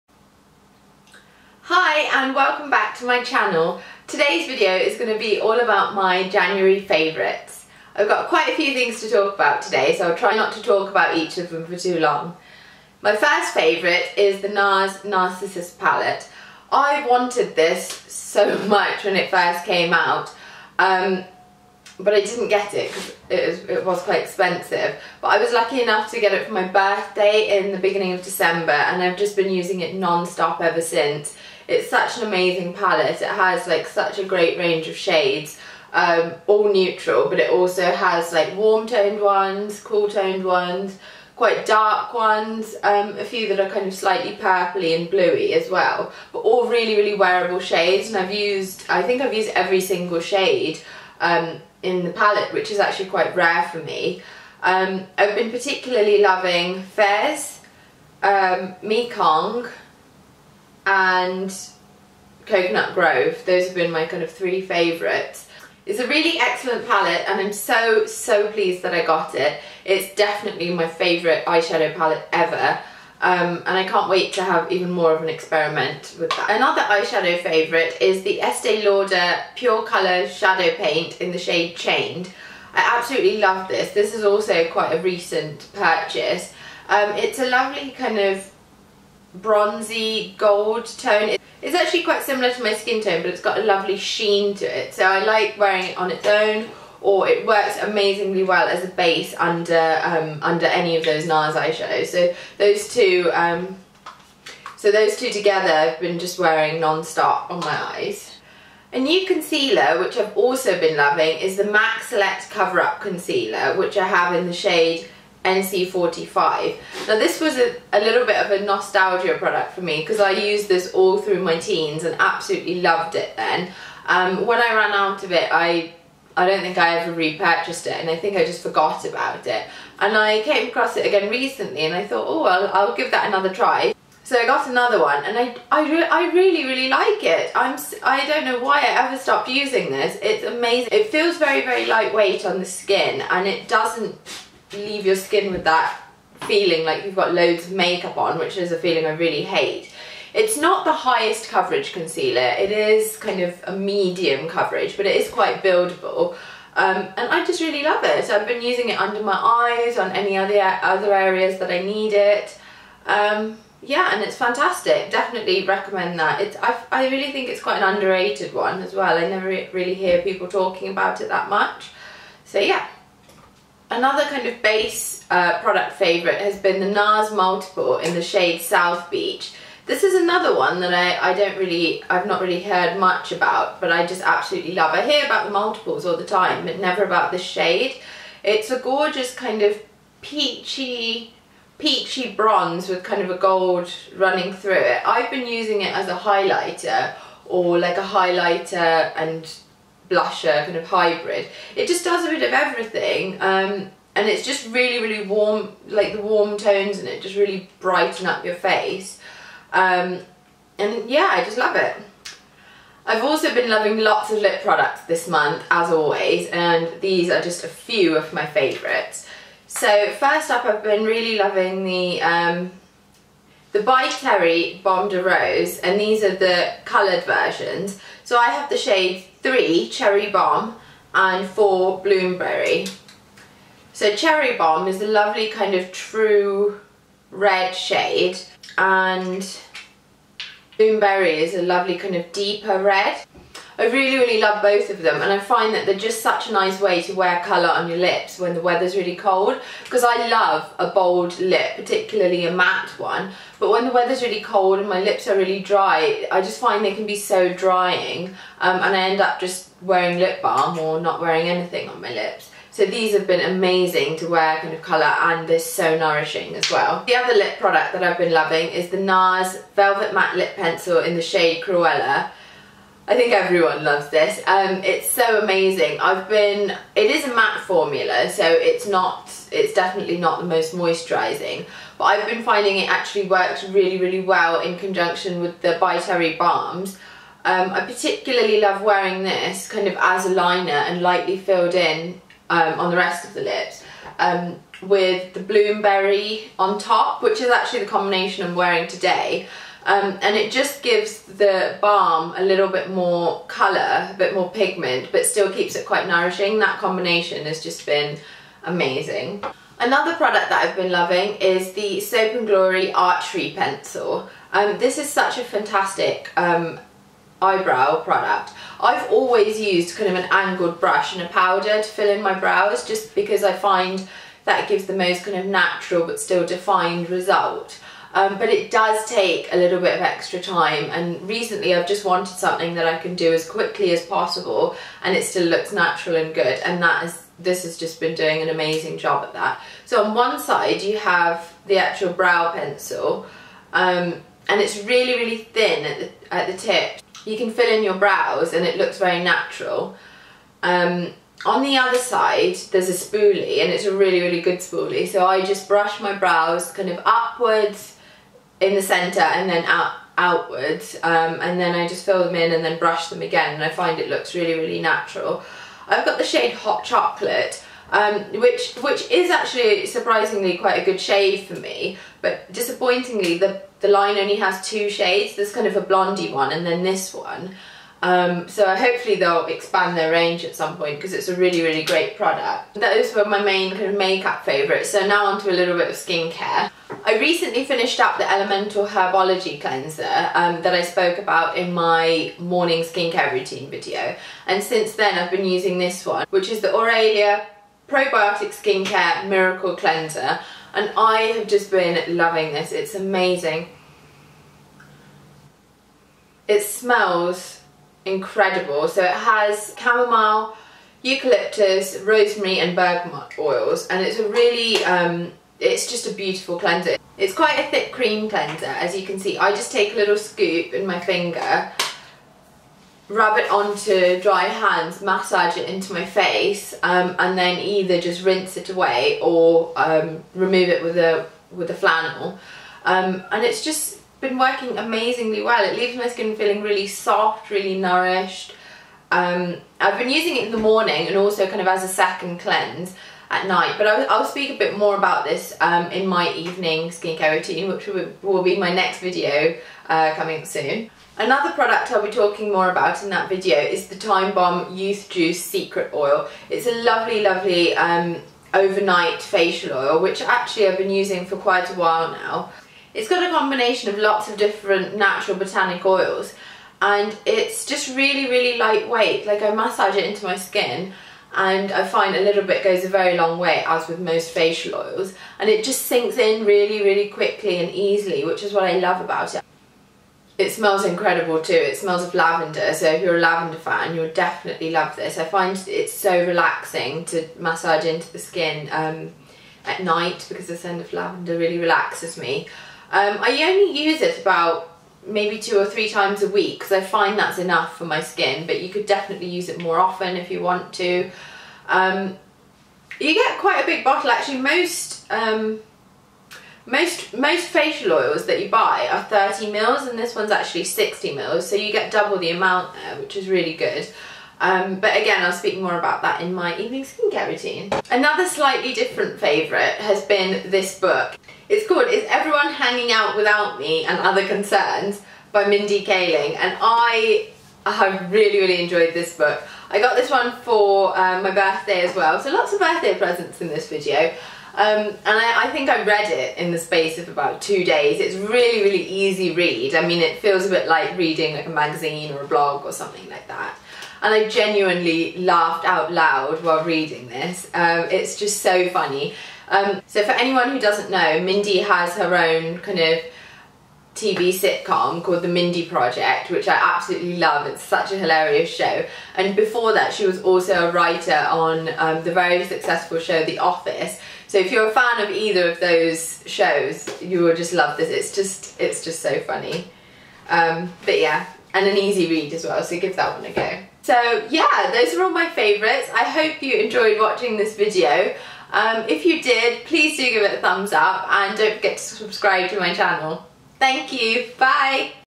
January Beauty Favourites
I hope you enjoyed hearing about my January Favourites!

Products mentioned:

Nars Narsissist Palette

By Terry Baume de Rose Nutri-Couleur in Cherry Bomb and Bloom Berry

Is Everyone Hanging Out Without Me and Other Concerns by Mindy Kaling